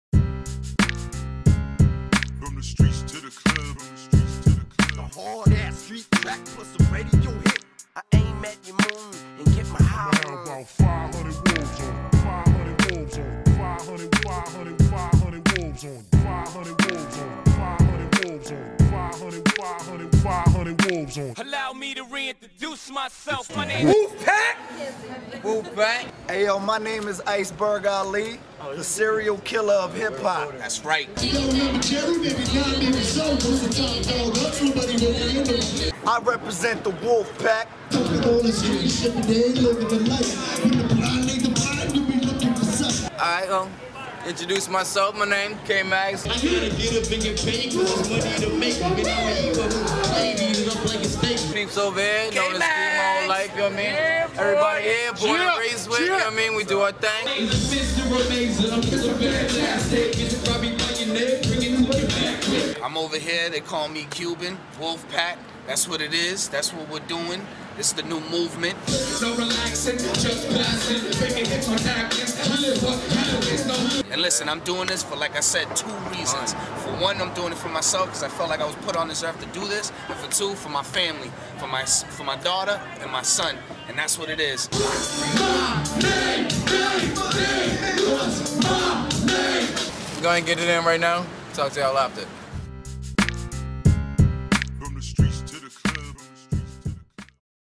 The Wolf Pack Intro
The Wolf Pack: Iceberg Ali, Cuban & K. Mags
- no. 9 ale house - malden, ma - 11/7/09